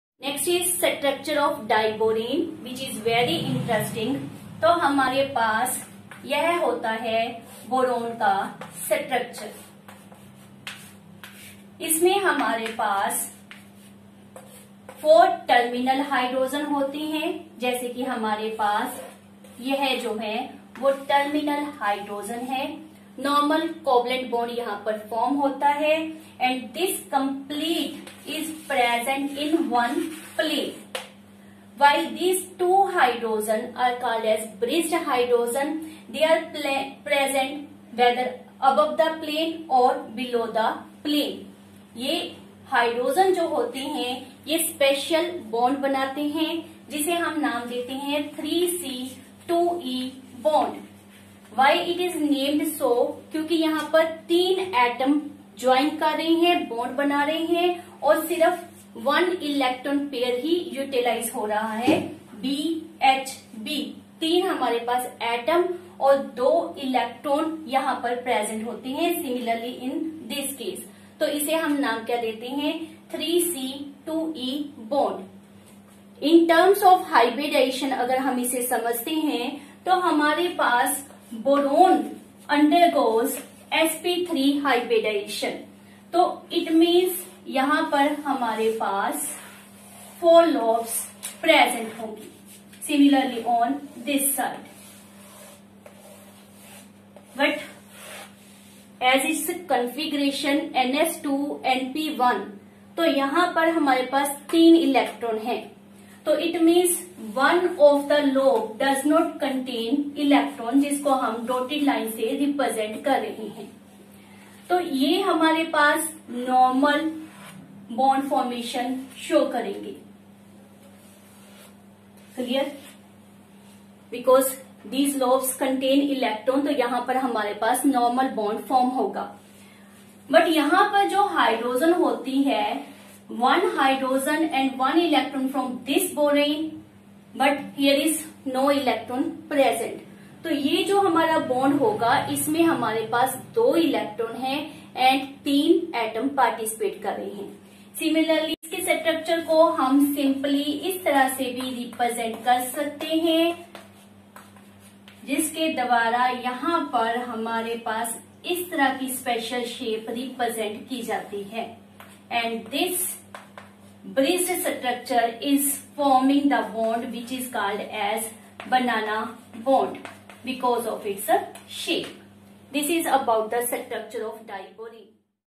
Structure and Reactions of diborane/class-11
In this video there is well explanation about the structure of diborane

 hybridization of diborane in structure

how the banana bond is formed is detailed explained here

formation of diborane

 heating effect on diborane

reactions of diborane in a given .